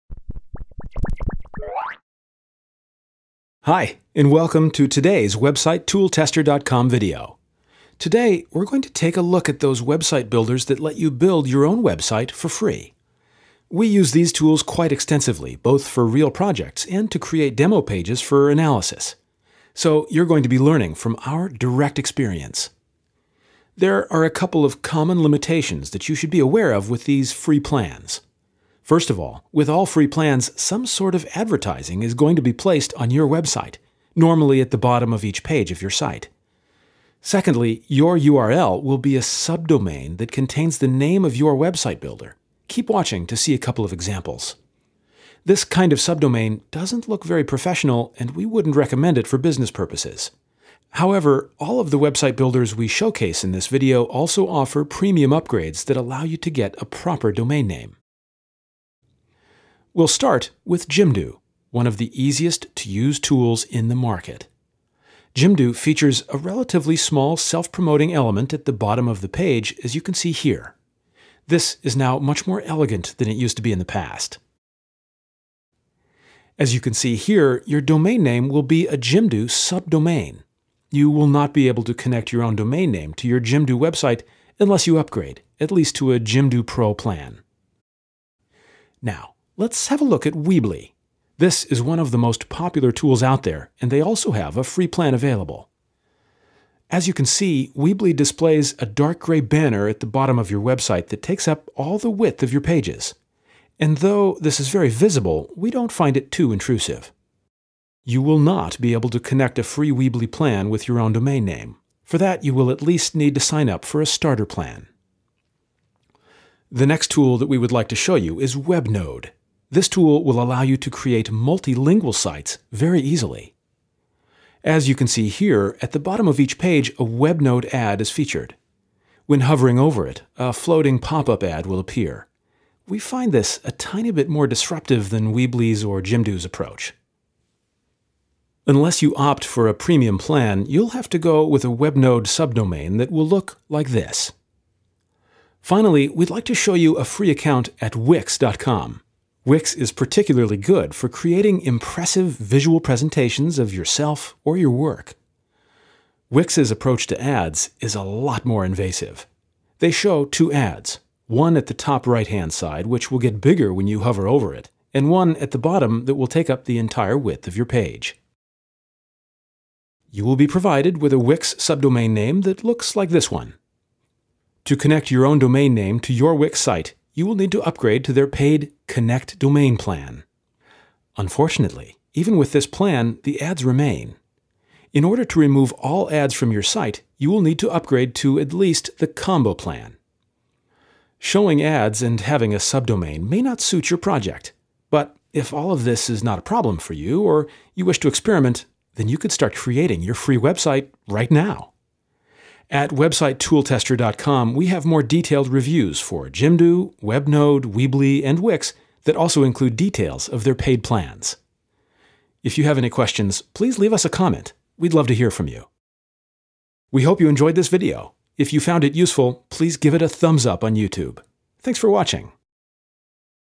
Free website builders: a comparison of the best ones
There are a lot of free website builders out there - but how can you find the best one for your website? Is it really possible to build and run a website for free with no ads? This video compares four different website builders: Weebly, Webnode, Jimdo and Wix.com

Content:
0:00 Intro
1:00 Jimdo
1:28 Weebly
1:57 Webnode
2:26 Wix
3:12 Conclusion